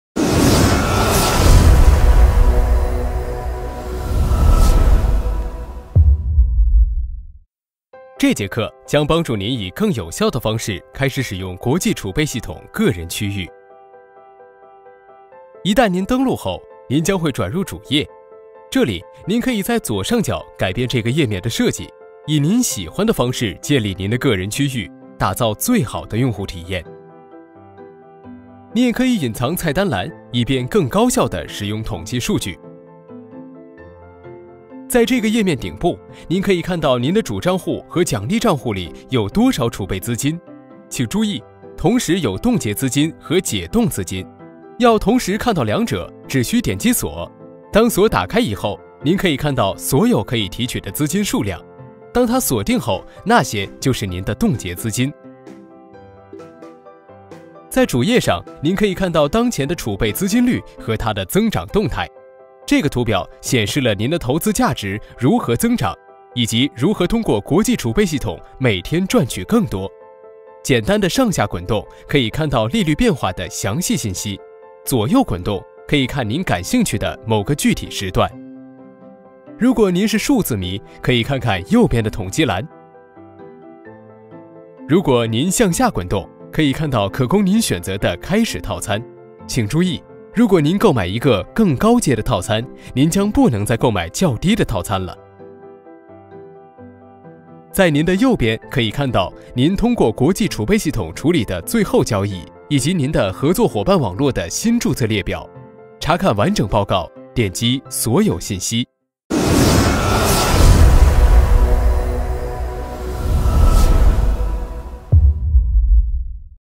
第8课 国际储备系统个人区域：利率，数据统计，套餐。
注意连续的故事或是它！

Watch out for the continuation of the story or be it!

国际储备体系是一个由一个目的联合起来的人的社区 - 建立一个使用比特币作为支付单位的替代金融系统。 根据创作者的意见，在新系统，信贷机构和国家的持续监控中没有财务控制的地方。 社会本身能够控制其行为，任何民主国家的宪法下的私有财产是不可侵犯的。 希望加入以色列开发商创建的国际储备系统的公民必须购买作为网络内付款方式的比特币。 进一步的加密货币被发送给其他参与者 - “Camerariuses”（现金持有人）作为“保留金钱”。
International Reserve System is a community of people united by one purpose - the construction of an alternative financial system using Bitcoin as a payment unit. According to the creators opinion there is no place to financial control in new system, credit institutions and ongoing monitoring by the state. Society itself is able to control its actions, and private property under the Constitution of any democratic state is inviolable. Citizens wishing to join the International Reserve System created by Israeli developers must purchase Bitcoins that serve as means of payment inside the network. Further on the crypto currency is sent to other participants - "Camerariuses"(Cash holders) as the "Reserved Money».
Ödeme birimi olarak Bitcoin kullanarak alternatif bir finansal sistemin inşaatı - Uluslararası Rezerv Sistemi tek bir amaç ile birleşmiş insanların oluşturduğu bir topluluktur. yaratıcıları görüşe göre devlet tarafından yeni sistemde, kredi kuruluşları ve sürekli izleme mali kontrol için hiçbir yer yoktur. Toplum kendisi hareketlerini kontrol edebilir ve herhangi bir demokratik devletin Anayasası kapsamında özel mülkiyet dokunulmazdır. ağı içinde ödeme aracı olarak hizmet Bitcoins satın almanız gerekir İsrail geliştiriciler tarafından oluşturulan Uluslararası Rezerv Sistemi katılmak isteyen vatandaşlar. "Ayrılmış Para" olarak "Camerariuses" (Nakit sahipleri) - Ek kripto birimi diğer katılımcılara gönderilir.
International Reserve System | Международная Резервная Система (IRS | МРС)